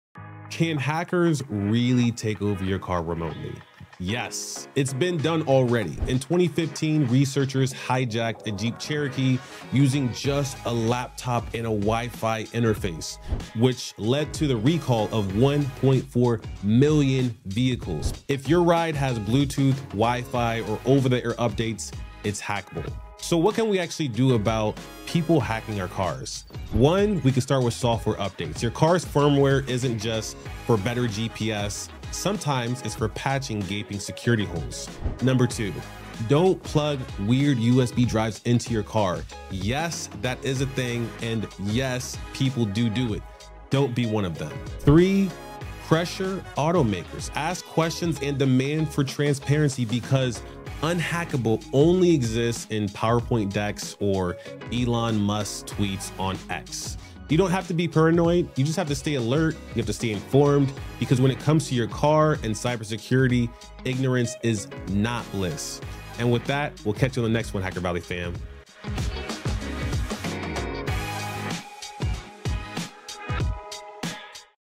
Your Car Is Hackable
Your car is hackable. Seriously. 🚗

Back in 2015, a couple of researchers turned a Jeep Cherokee into a remote-controlled rollercoaster: cut the brakes and hijacked the steering, all from a laptop. So what about today’s vehicles? Still vulnerable.

Ron breaks down the risk of car hacking, why automakers are scrambling, and how to drive safer in a world where wheels meet WiFi.

Should firewalls come standard in new cars, or are we still trusting infotainment systems that lag when you skip a song? Let us know what you think! 💬